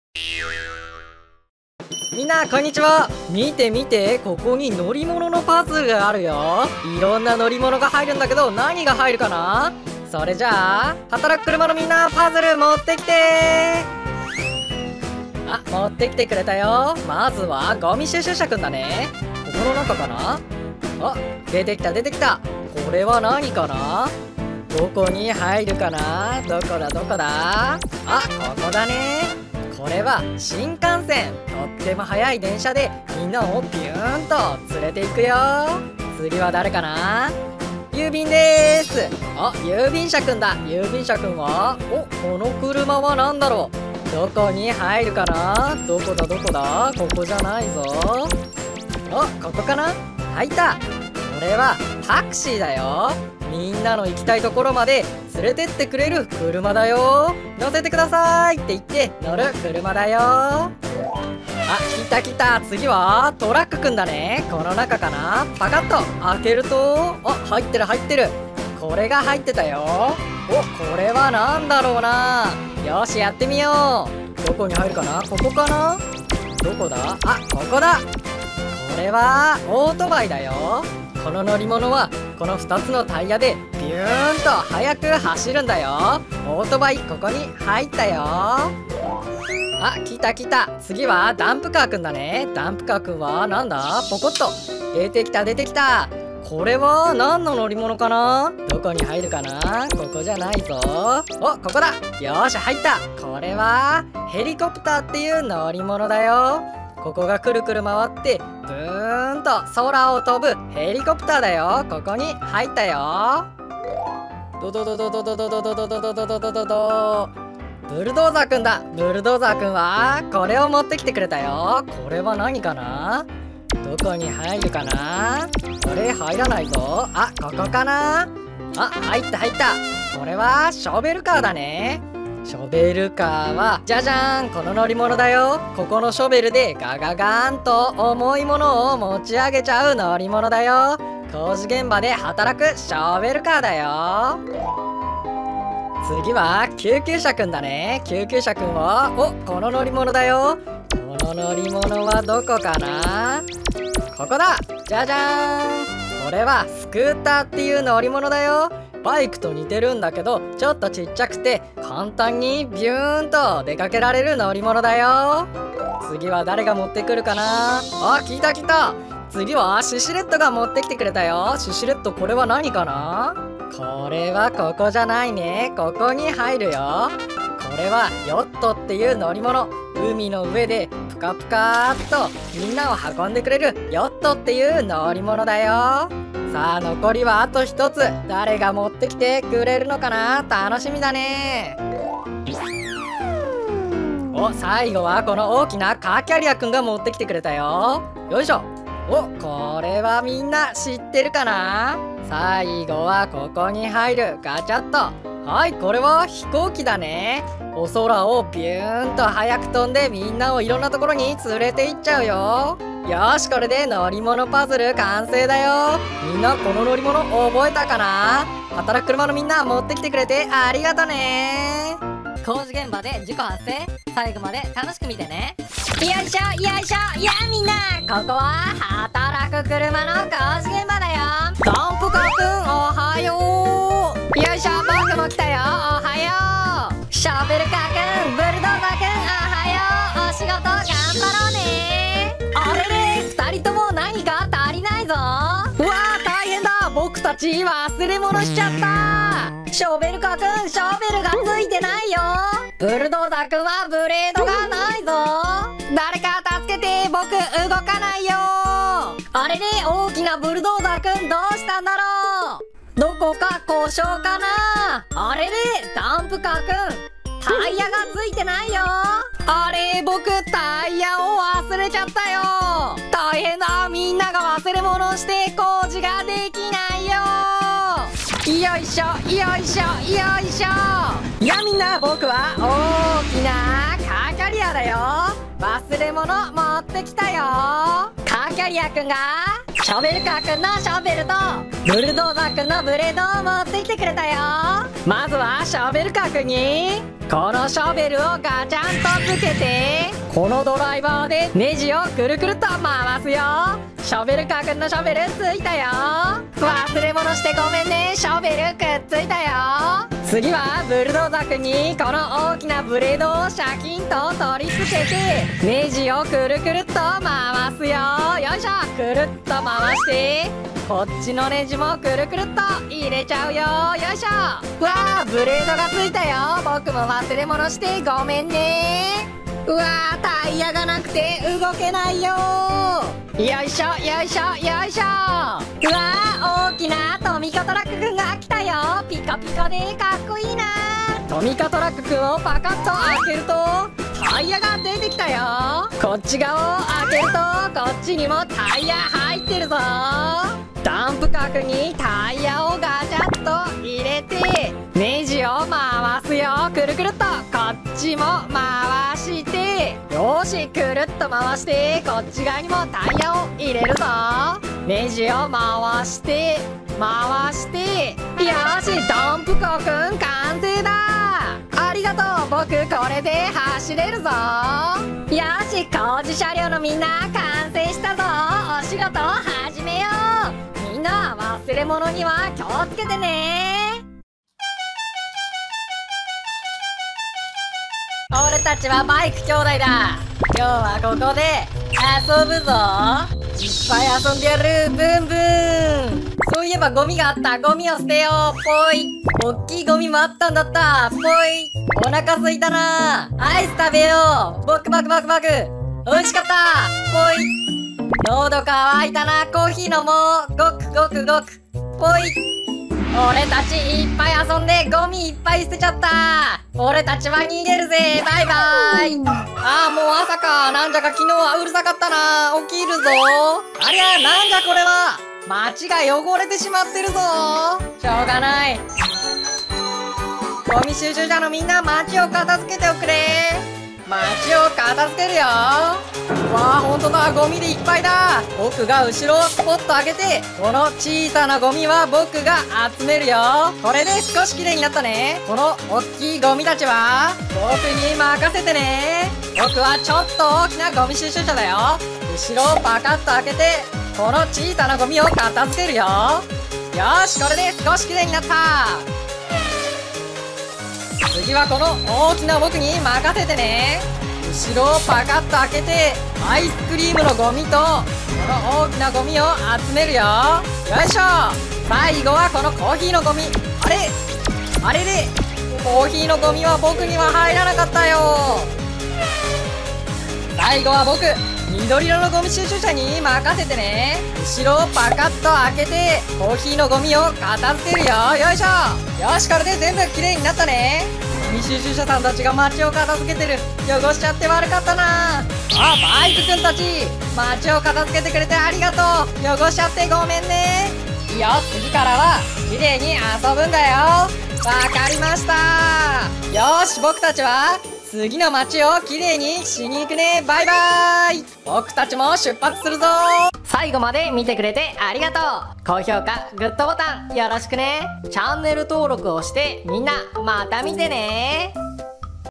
はたらくくるま のりものパズルで乗り物の名前を覚えよう♪ ごみ収集車 救急車 ショベルカー ブルドーザーなどが登場☆
あんどといずの再生リスト☆